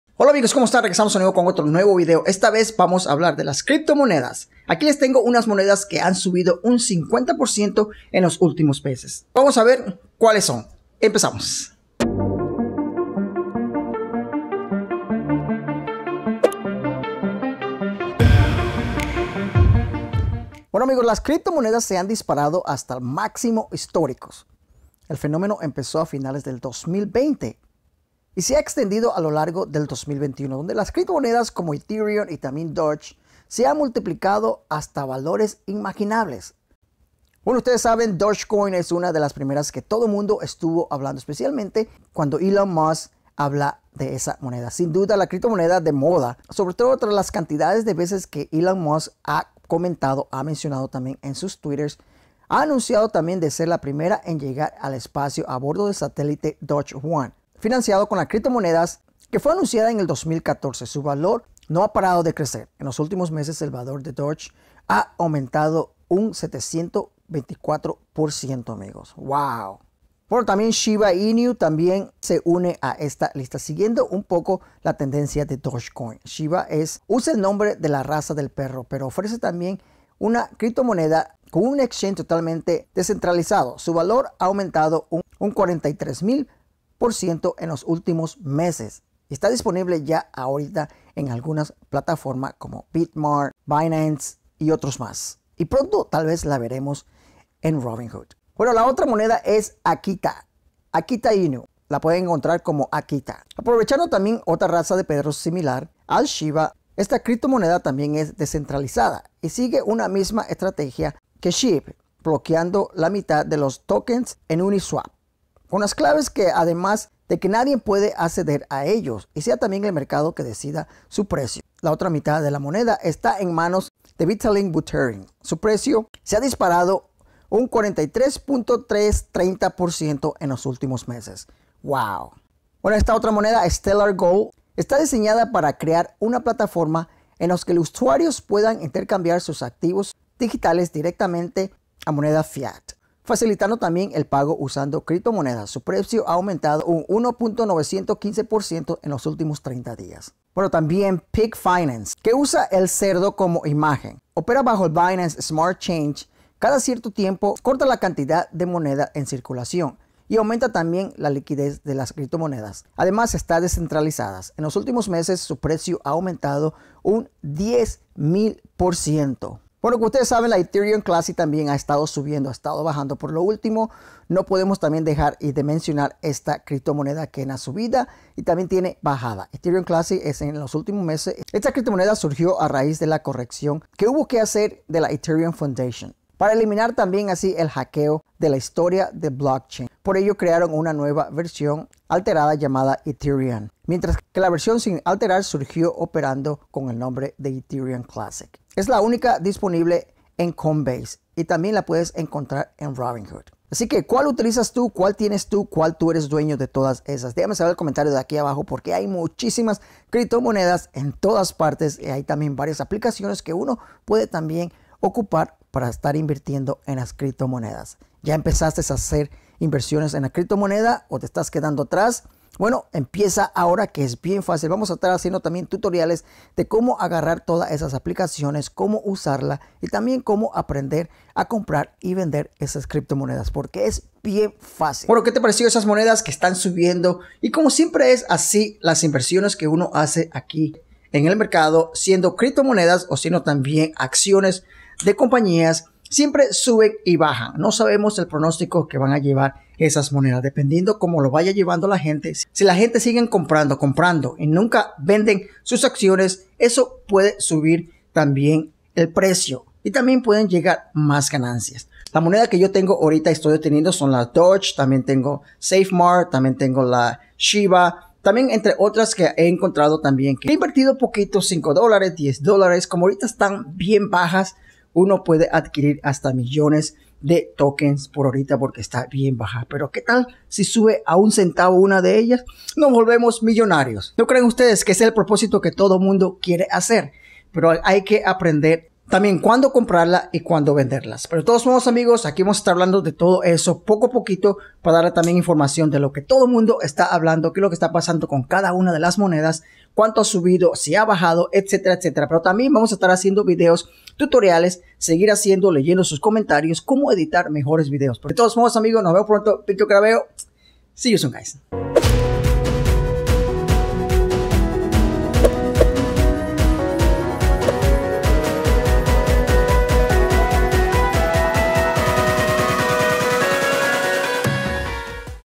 Estas CRIPTOMONEDA han subido hasta un 44.000% en el último MES Shiba Inu - Dogecoin y otras MAS
quieres más videos de las criptomonedas solo deja tu comentario..